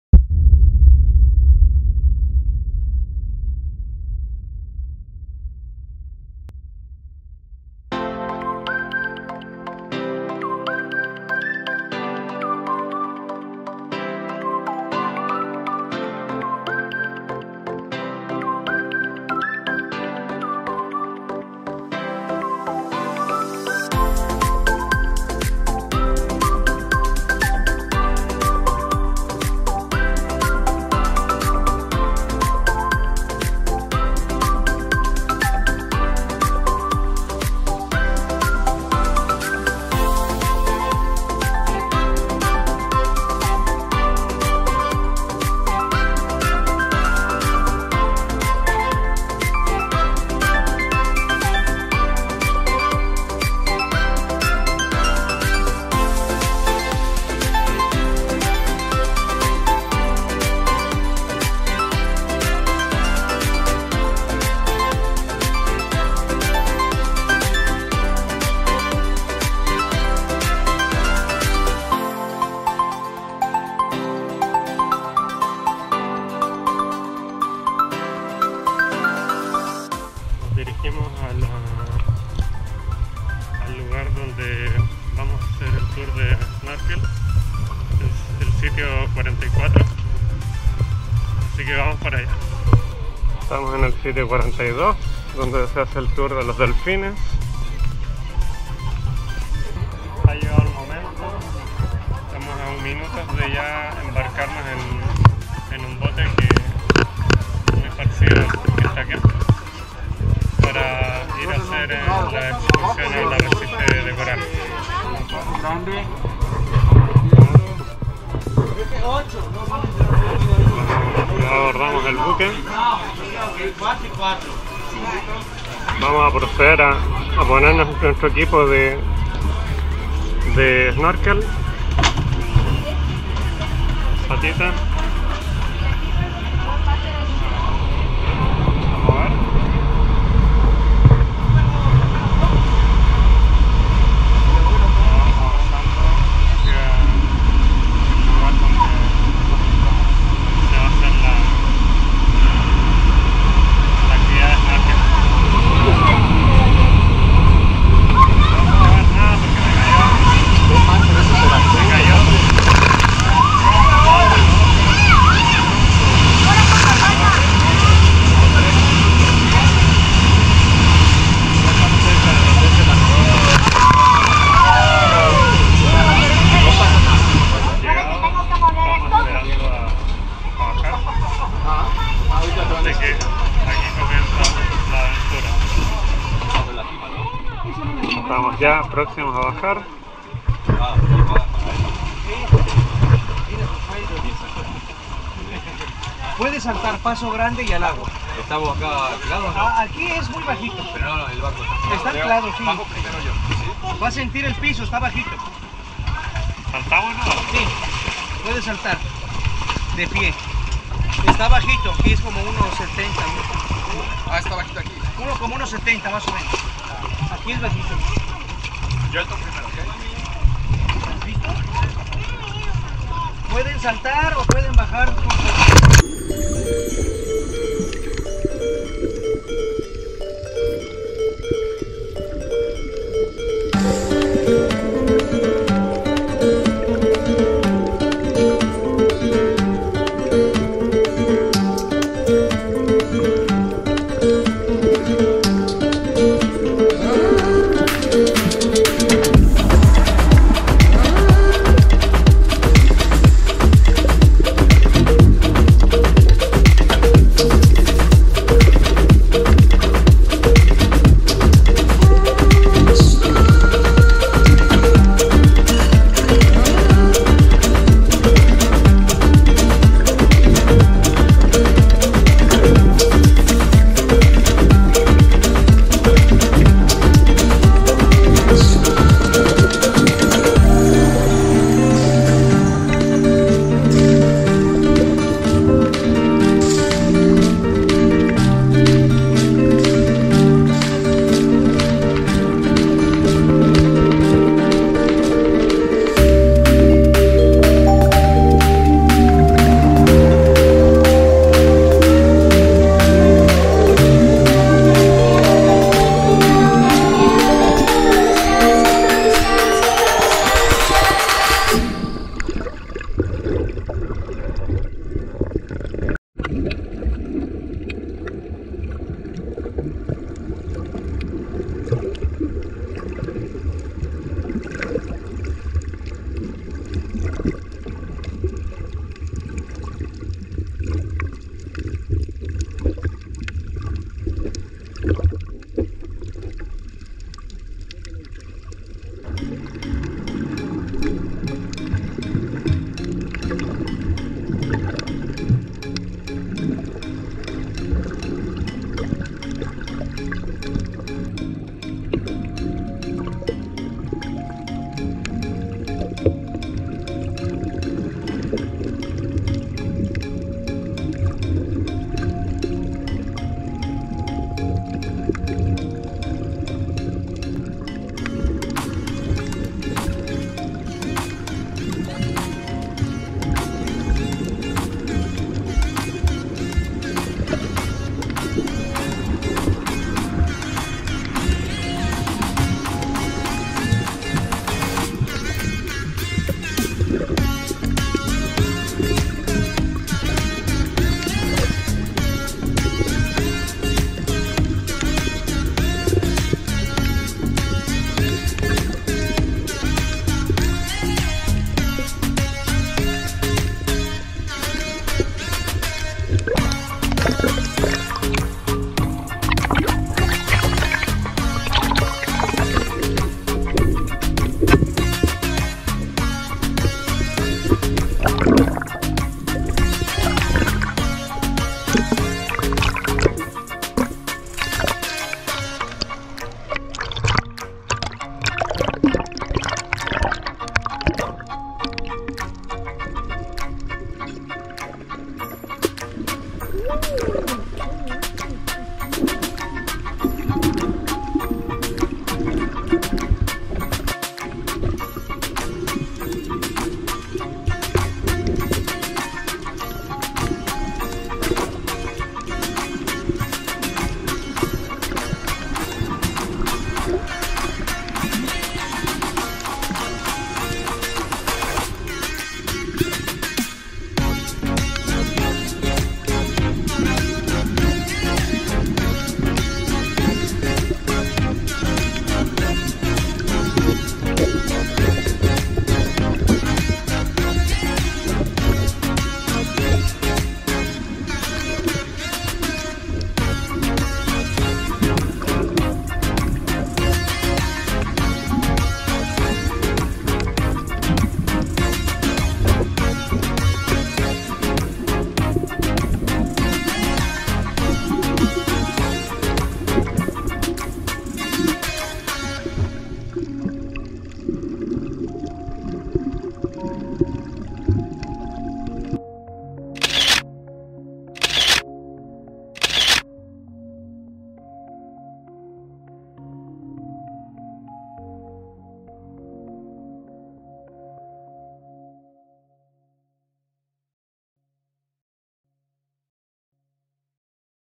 Arrecife Barracuda - Xcaret by México - Chocando con Arrecifes de Coral
Hoy nos sumergimos en las profundidades del Mar Caribe para vivir una aventura extraordinaria por los arrecifes de coral circundantes, en una excursión que les mostrará la vida marina del lugar, como peces tropicales, manta rayas, corales, algas marinas y alguna que otra sorpresa.